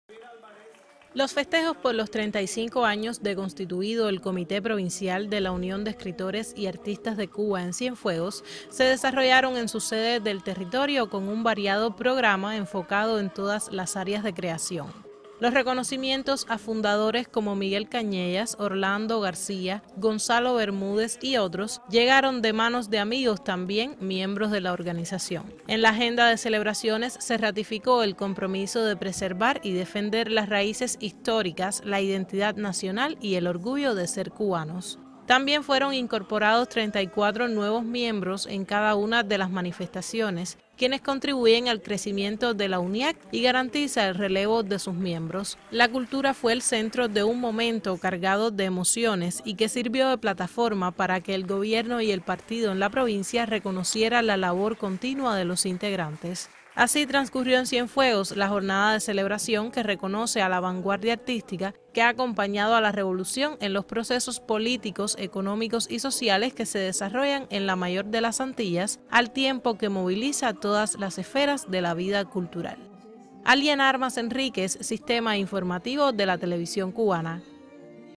Celebra Comité Provincial UNEAC Cienfuegos aniversario 35 de fundado
El Comité Provincial de la Unión de Escritores y Artistas de Cuba en Cienfuegos festejó su aniversario 35 de creado. En el encuentro recibieron el homenaje miembros fundadores y se entregó a los nuevos ingresos el carné que los acredita como miembros de la organización.